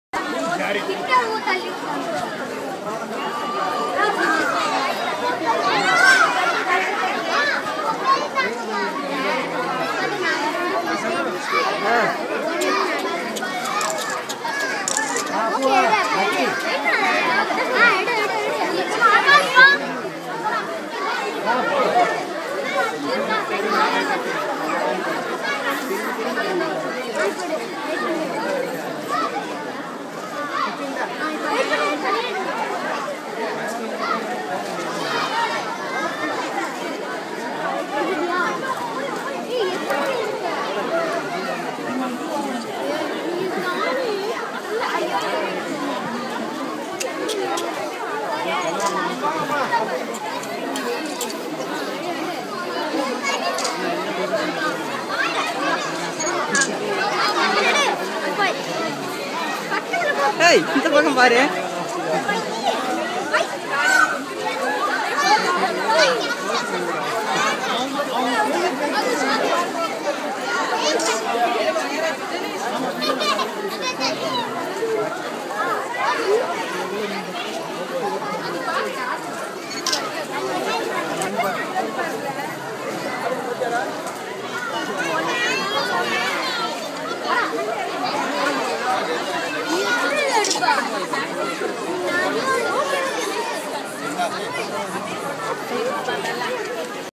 White Peacock Dance :: White Peafowl Dance [HD] - Raamesh Keerthi
Dancing Peacock @ Vandalur Zoo...